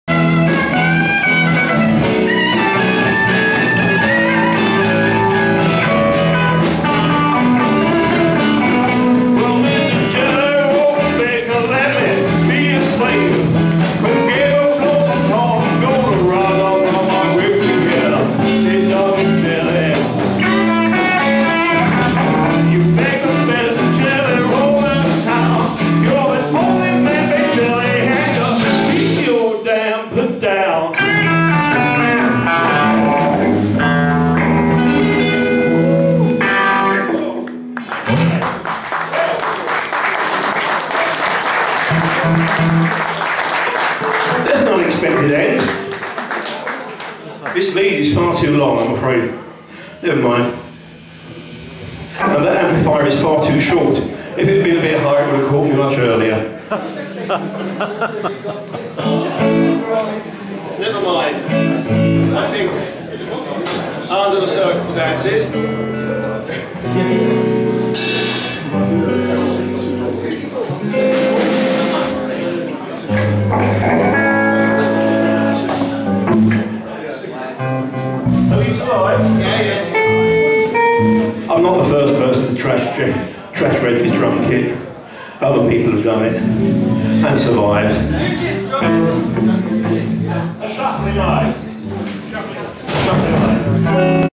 PLANK SPANKER OVERWHELMED BY APPLAUSE AT THE C&H 11/4/12
PLANK SPANKER STEVIE KING PUSHED OVER BY AUDIENCE APPLAUSE  ALSO TESTING  HIS AMP AND  REG PATTERNS DRUM KIT